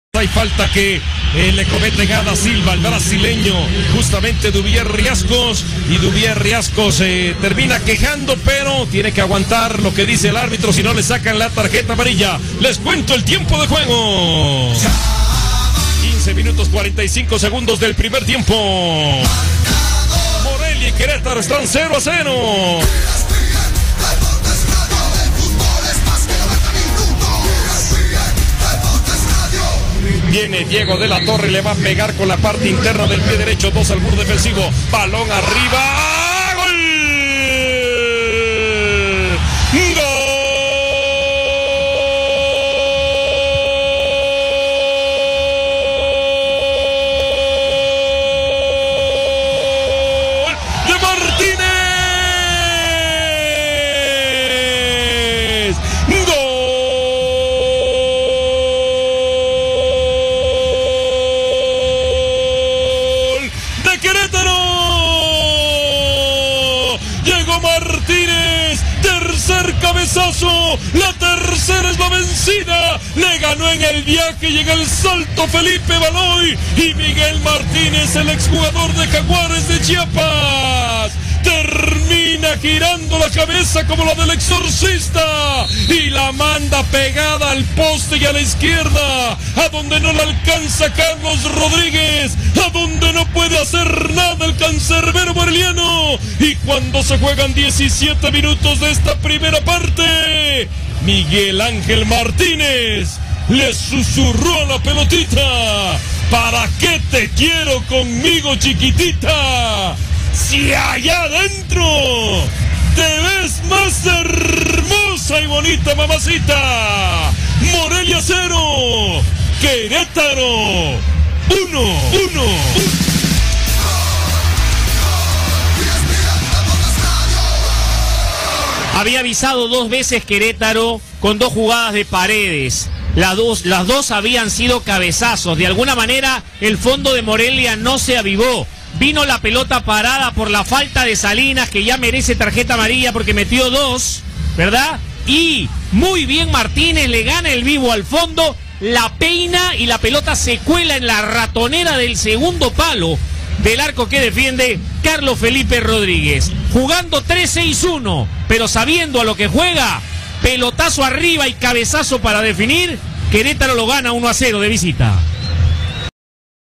Monarcas Morelia 0 - 1 Querétaro (Narración ESPN Deportes Radio)
El primer gol del Clausura 2014, en ESPN Deportes Radio con el relato de Dionisio Estrada.